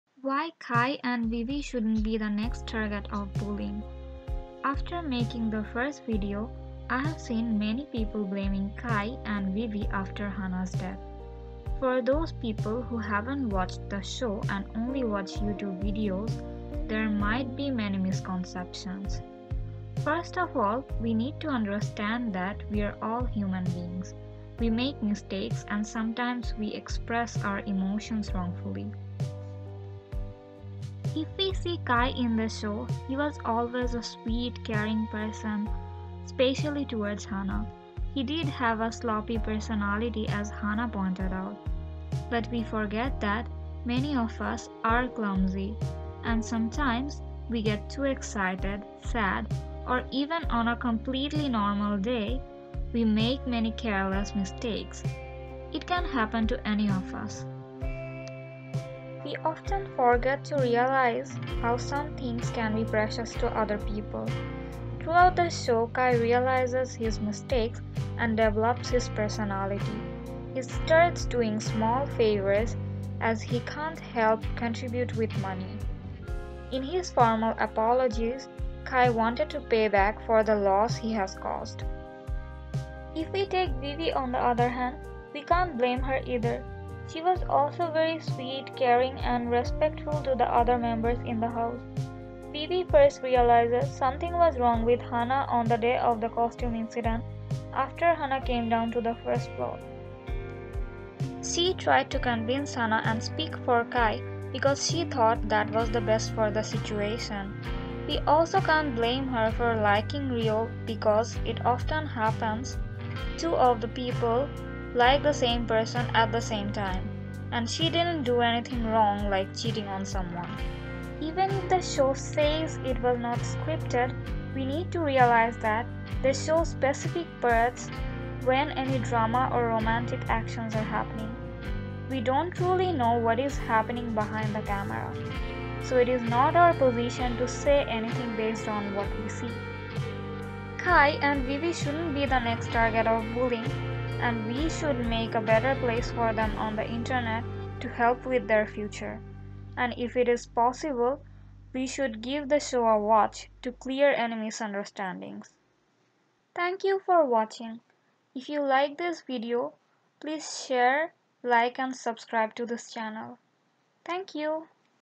Why Kai and Vivi shouldn't be the next target of bullying.
Some of the people are blaming Kai and Vivi after Hana's death. This video is made so that they don't get bullied and blamed for the matter and everyone tries to understand their point of view. Kai and Vivi are wonderful people and we should make their future safe.